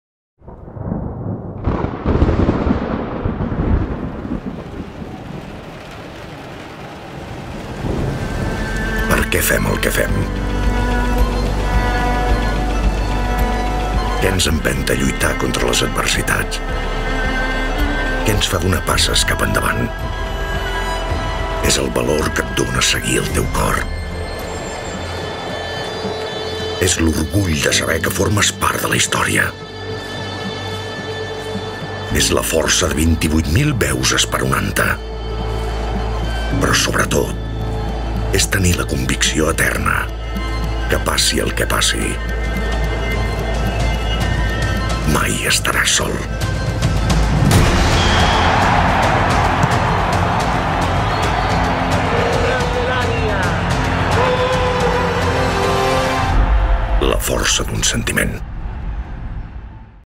RCD Espanyol | Preguntas (2020)
2020 Spain
Client: RCD Espanyol de Barcelona
Agency: AMT Comunicación
Production: Kiwi Film

La Força d'un Sentiment
Campaña de abonos temporada 2020/2021
With: Dídac Vilà, Denis Suárez

Football Ads